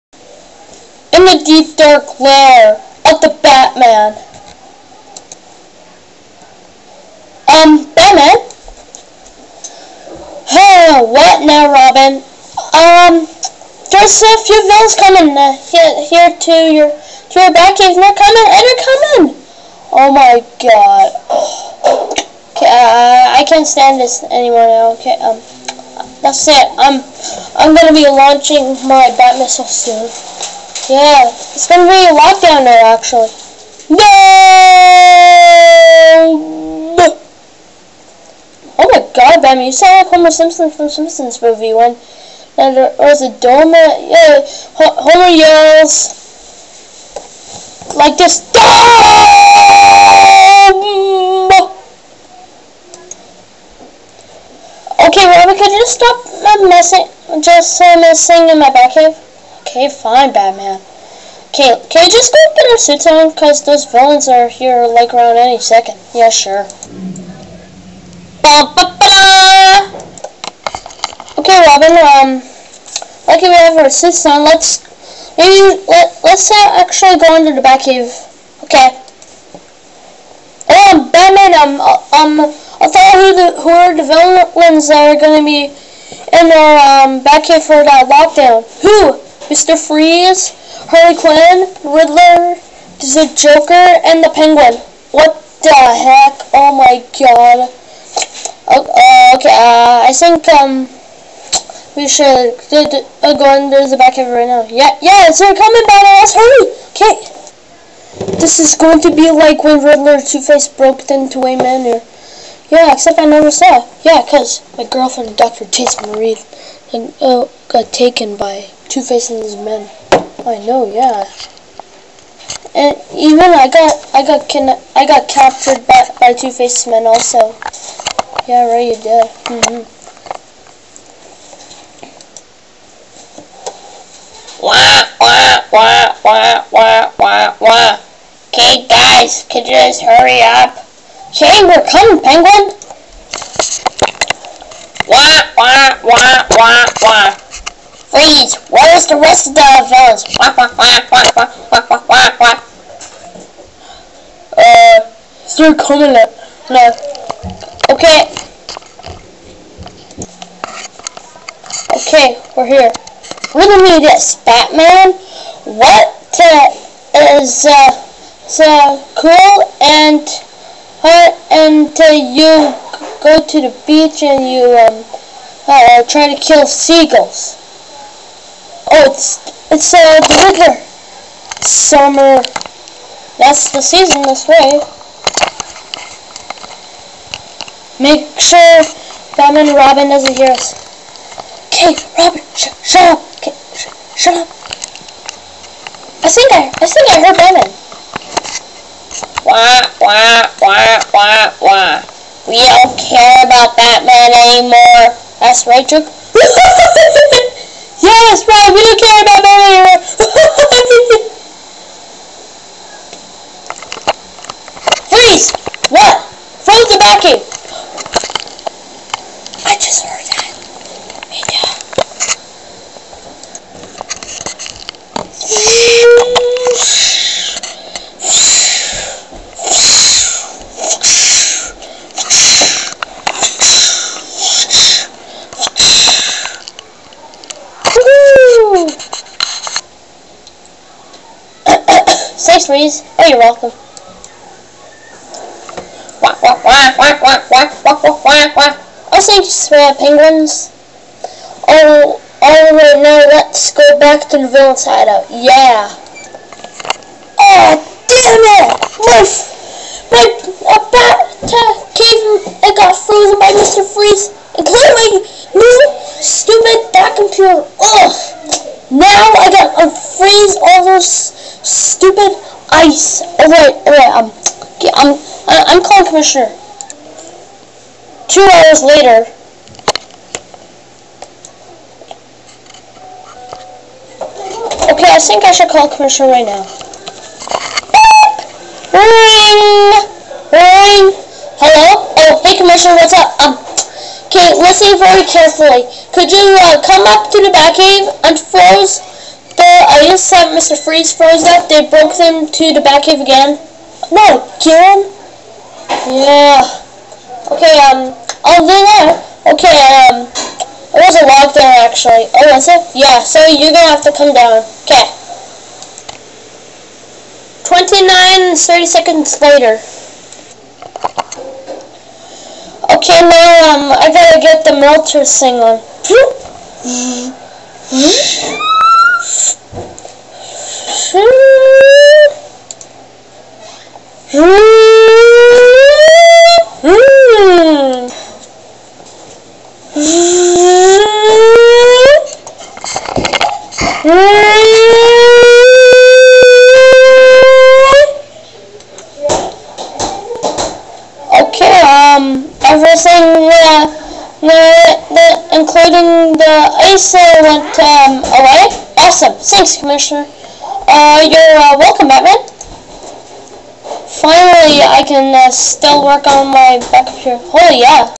Lego Batman Episode II,Lockdown
Lego Batman and Lego Robin just stays in the Batcave for a Lockdown and Mr.Freeze Frozes up the Batcave and The Riddler tells Batman a Riddle and After When the Villians were Gone Batman Notices the Batcave has been Frozen so he had to Call Comissioner Gordon to Unfreeze the Batcave.

Note: If You're Going to Comment don't Say at 2:14 - 2:43 is Cowder.